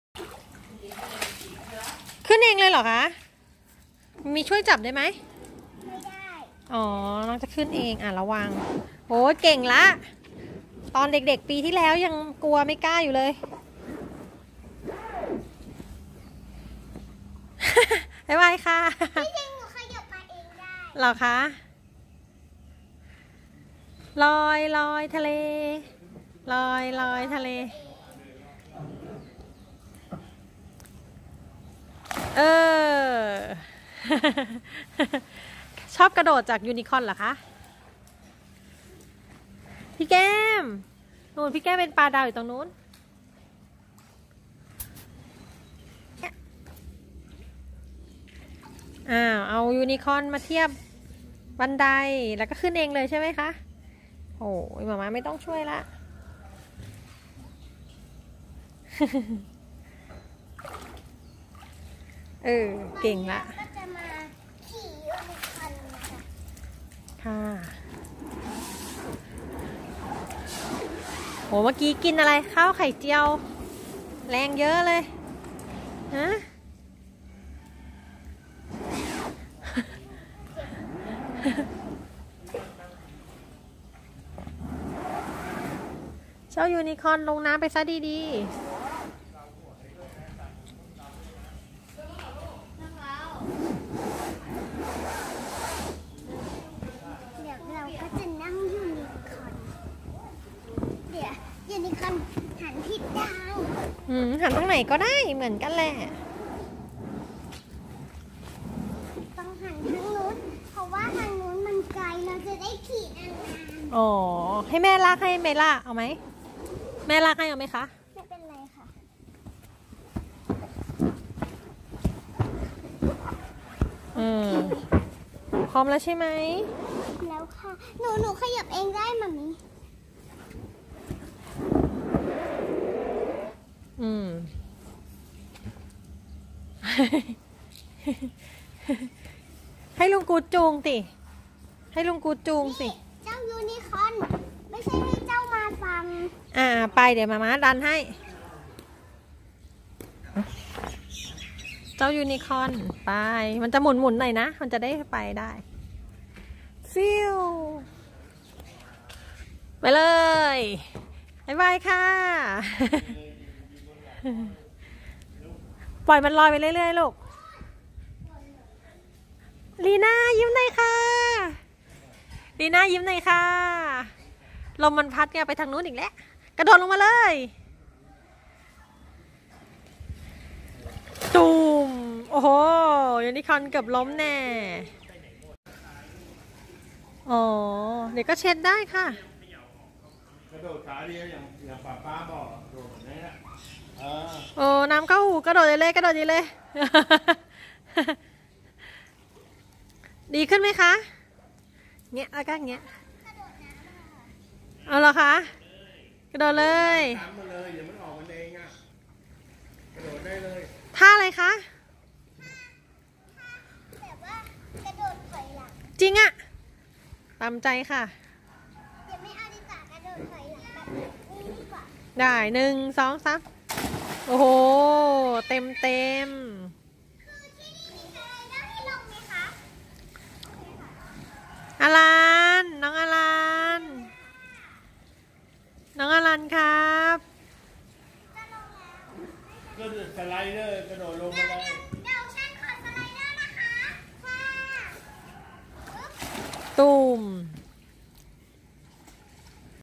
รีน่า อริญรดา กระโดดลงน้ำจากยูนิคอร์น
4พี่น้องมาเที่ยวด้วยกัน มีความสุขที่สุด
rinaarinrada 5y5m. AlanKritayos 3m20d.